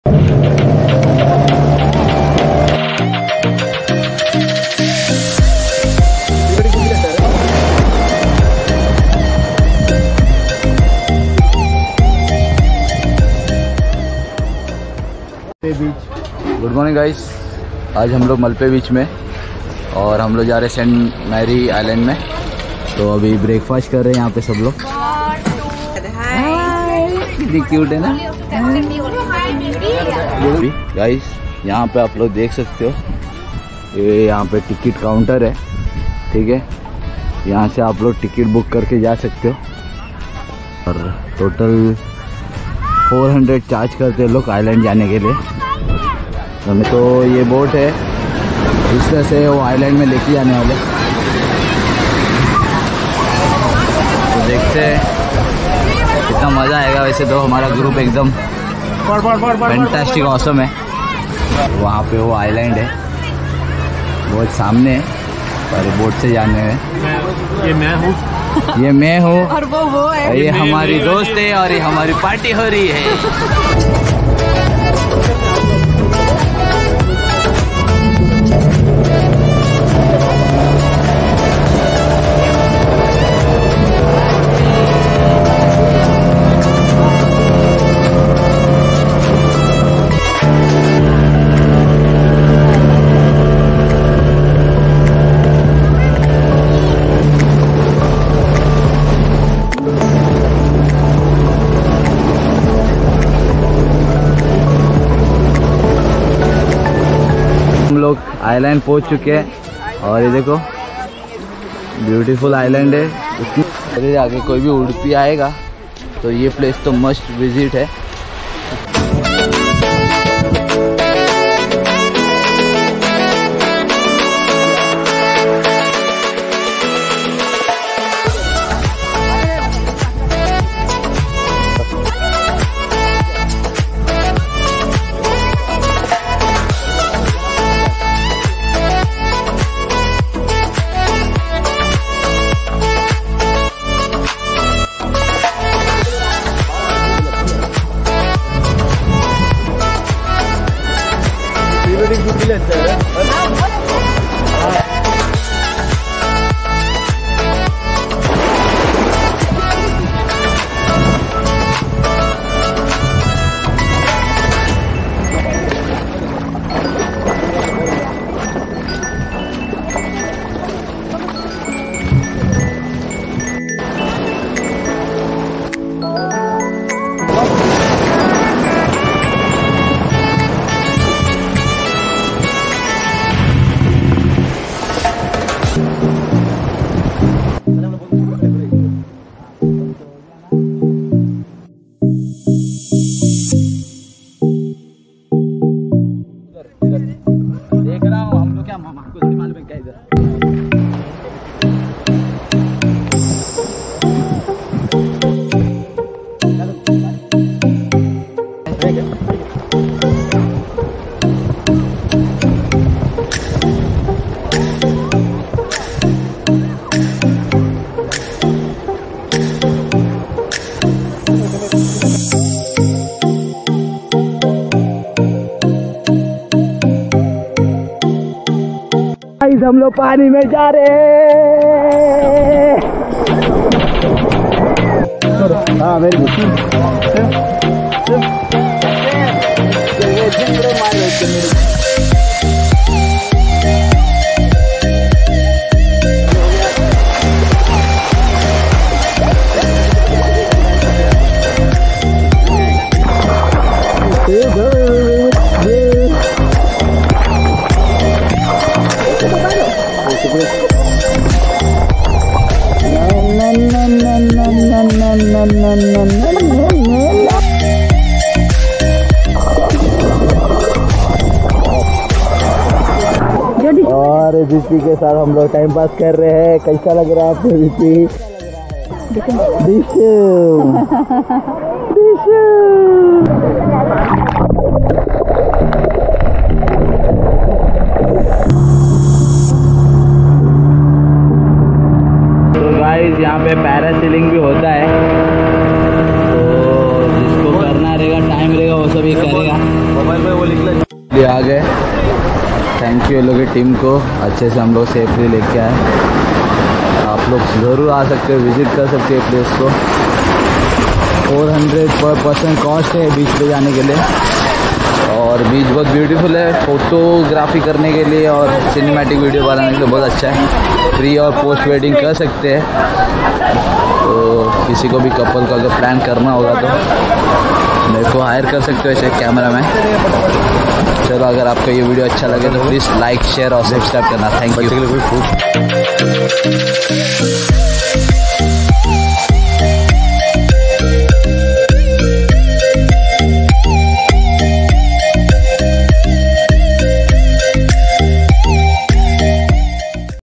vlog7 | St Mary's Island | Udupi Tourism | Hidden gem in Karnataka | Best place | Blue water 😍
St. Mary’s Islands, also known as Coconut Island in Udupi, Karnataka, India.

Mini Maldives with its unique rock formations that cannot be found anywhere else in India and for brilliant sea views.

It is a touristy island with adventure sports and sight seeing around the island.

It was unforgettable and amazing experience😍🏝️.

The only way of getting to the islands is by boat. Booking office is located at Malpe Beach, Opp to Paradise Isle Resort Near By Gandhi Statue .

It's cost 400 rupees per person to visit this island . You can directly visit malpe beach there you will find a booking office  to book tickets .

Visiting timing is from morning 8 am to evening  4 pm

shoot cinematic videos with using Insta 360 one X2 & GoPro Hero 9.

Hit  *LIKE* if you enjoy this video & *COMMENT* ur thought.

*The original copyright(s) is / are solely owned by companies / original artists / record-label.*